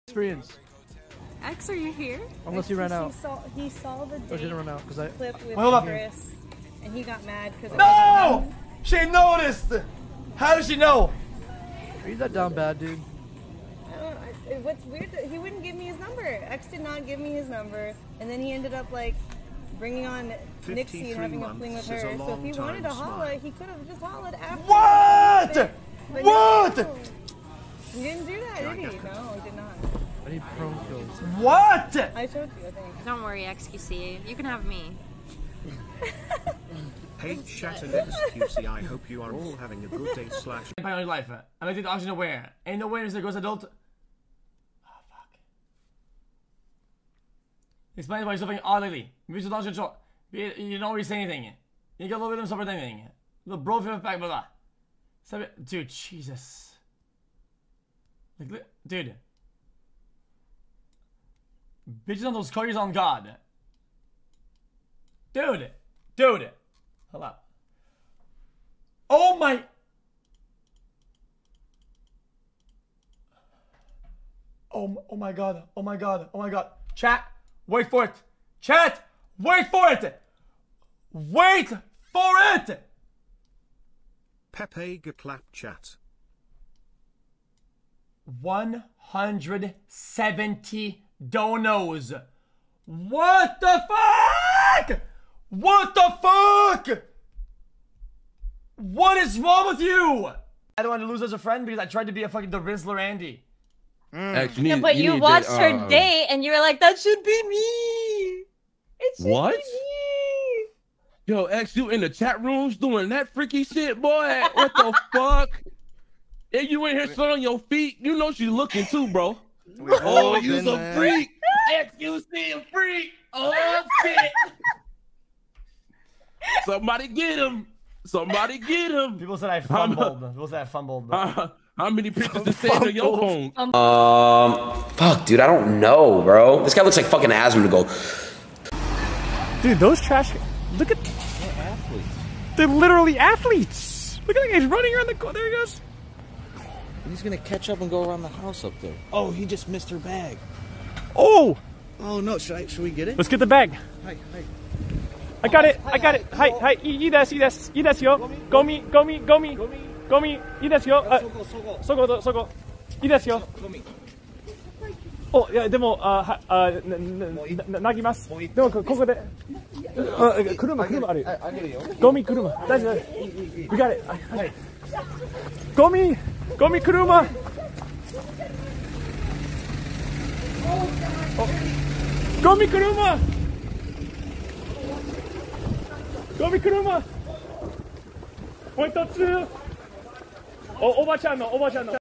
Day 329 - Just Chatting - Top 5 Twitch Highlights 2022
00:00 - xQc fumbled the bag

00:40 - -$850

01:40 - xQc caught in 4K

02:13 - Adin finds Asmongold

02:18 - Helping out oba-chan with the trash